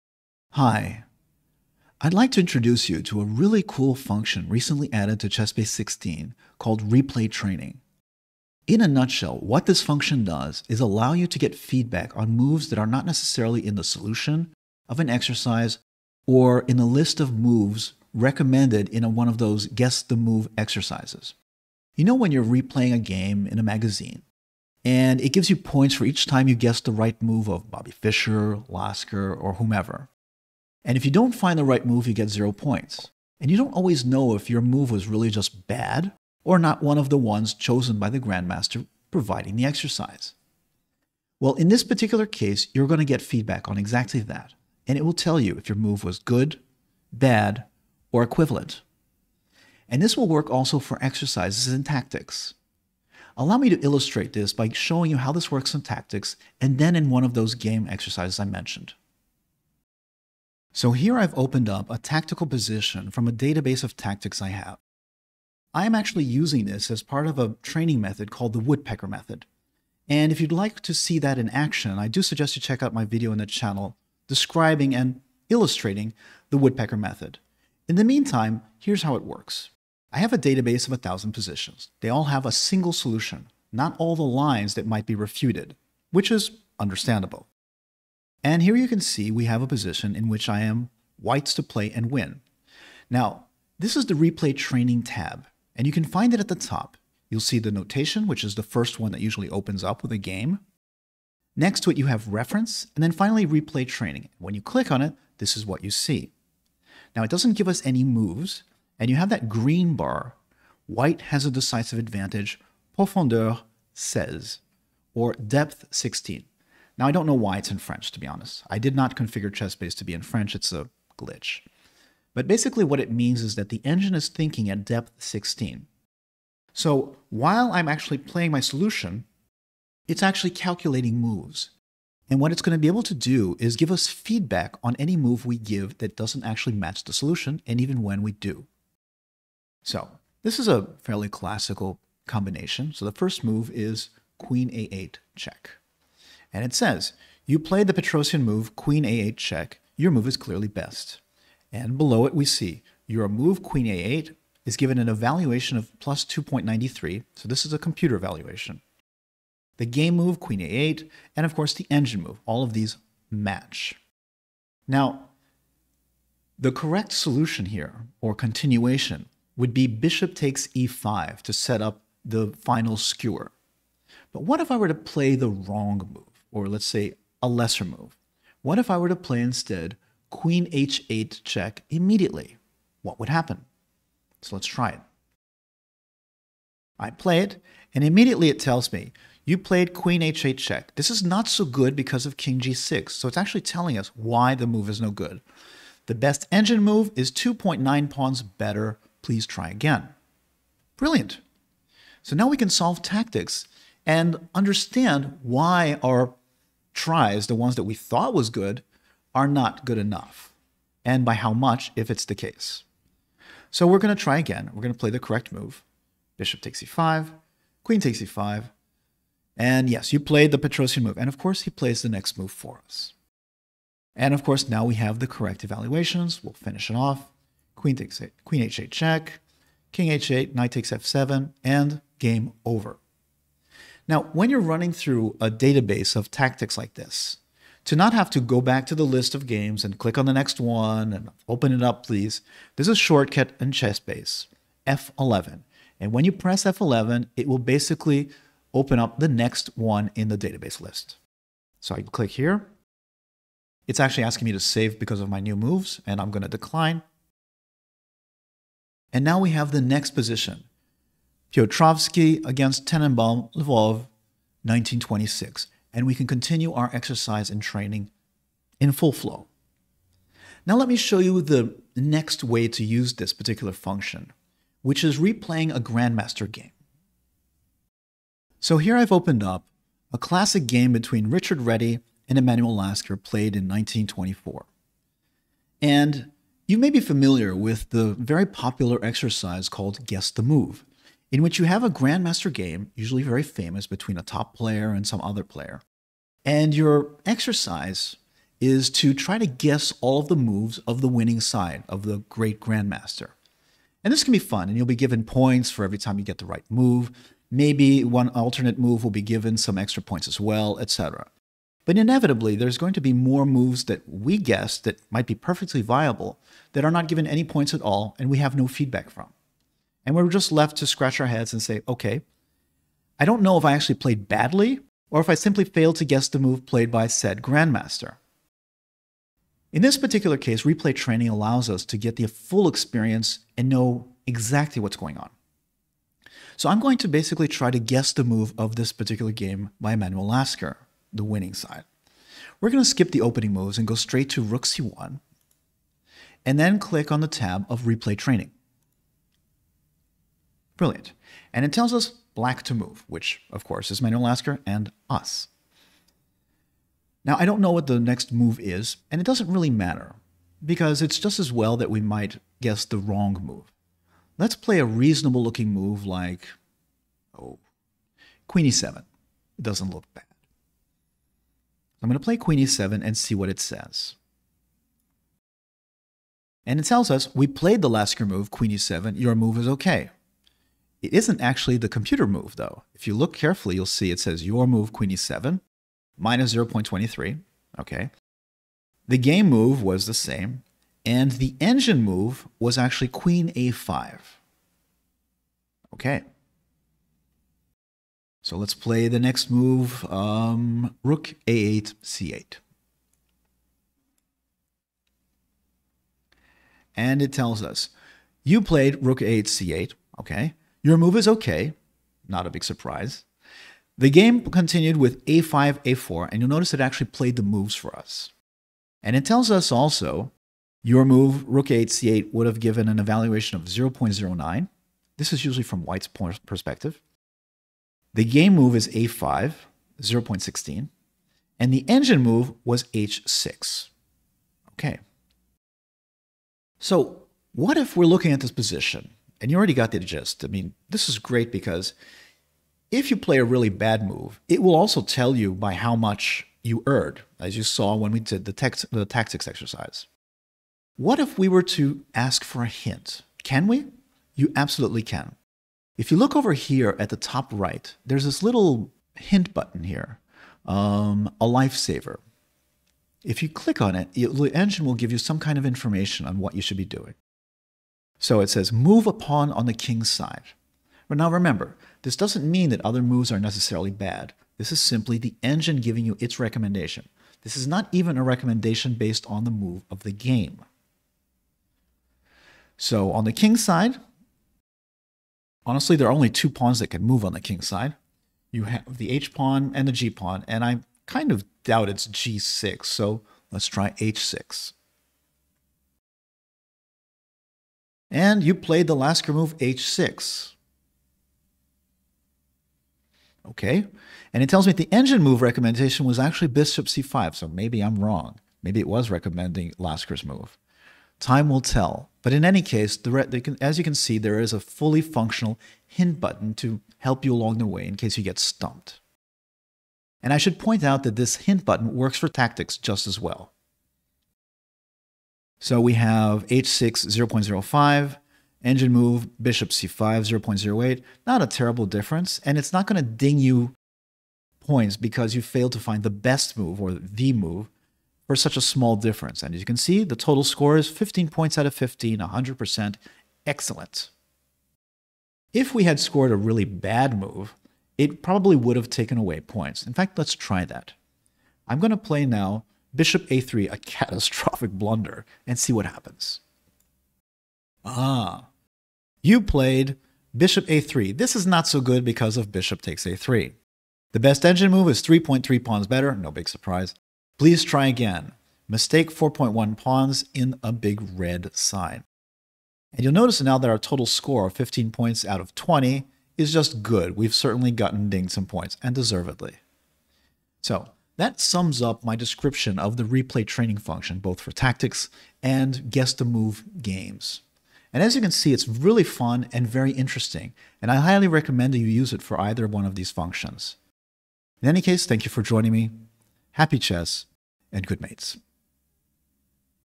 ChessBase secrets - Replay Training, a new hidden gem!
ChessBase is a program in constant evolution, and even the version you buy will enjoy not just updates and big fixes over time, but may see powerful new functions added as well. Here is an overview and tutorial of a new function called Replay Training that can be a godsend for tactics training or fun exercises such as Guess The Move, also known as Solitaire Chess in Chess Life magazine. Don't miss out on this new feature!

Chapters:
00:00 Intro
00:59 Using replay training with Tactics
04:38 Replay Training in Solitaire Chess (Guess the move)